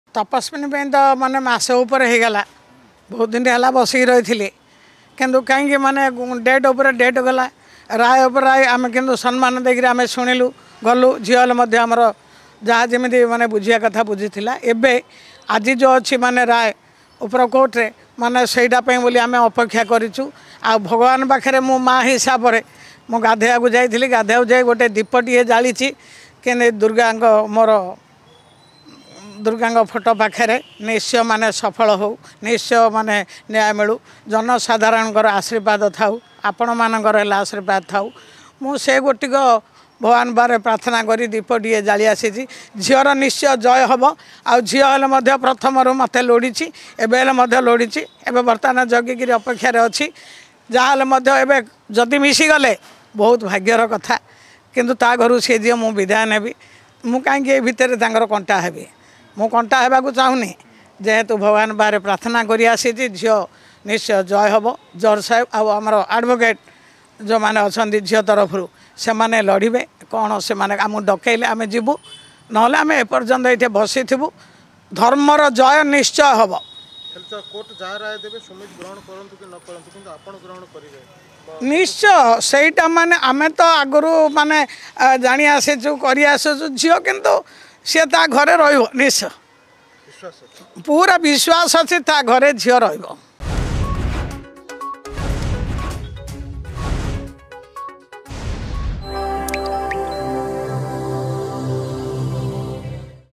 ଡିମ୍ପଲ ଏବଂ ସୁମିତଙ୍କ ଏକାଠି ରୁହନ୍ତୁ : ପ୍ରମିଳା
ଆଜିର ରାୟ ପାଇଁ ଆମେ ଅପେକ୍ଷା କରିଛୁ । ମା' ହିସାବରେ ମୋର ଭଗବାନଙ୍କ ପାଖରେ ପ୍ରାଥନା ଡିମ୍ଫଲର ଜୟ ହେଉ ।  ଡିମ୍ପଲ ଏବଂ ସୁମିତଙ୍କ ଏକାଠି ରୁହନ୍ତୁ : ପ୍ରମିଳା